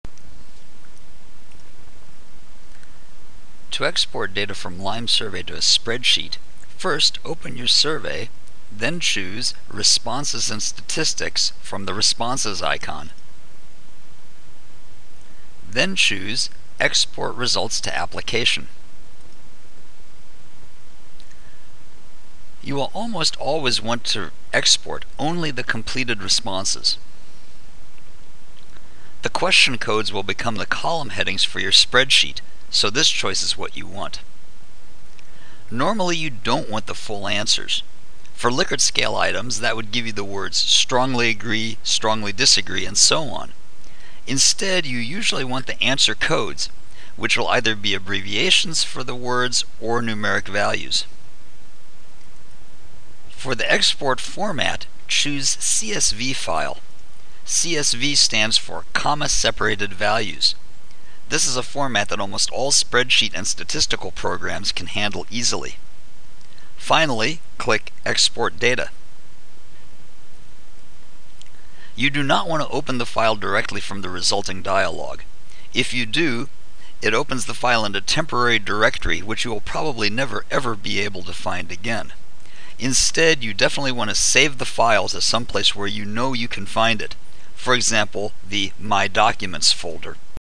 Exporting Data from LimeSurvey
How to export data for use with SOFA statistics or R